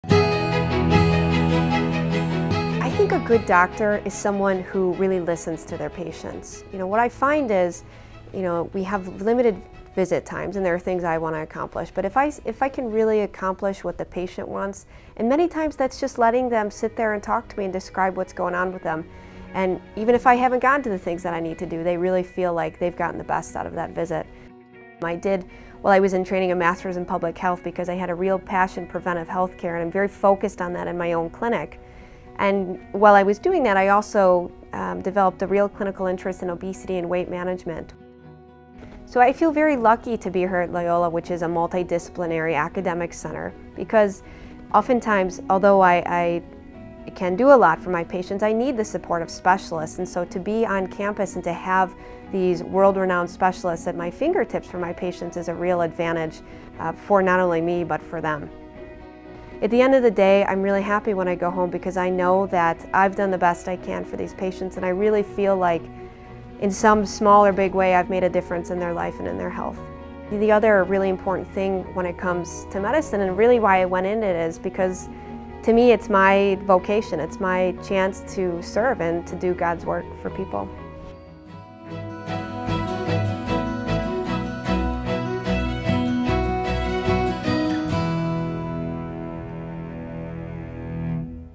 Internist: Monica Edwards, MD
Dr. Monica Edwards, a primary care physician, really listens to her patients and why they decided to make an appointment. Besides earning her MD, Dr. Edwards also completed a Master's in Public Health because she has a special interest in preventive health care. During that time she became particularly interested in obesity and weight management. She really appreciates the advantages she has as a primary care physicians at an academic medical center with nationally renowned experts. Whenever she needs to consult with a specialist, they are right here on campus. She feels a great deal of satisfaction in practicing medicine and is very happy to serve others.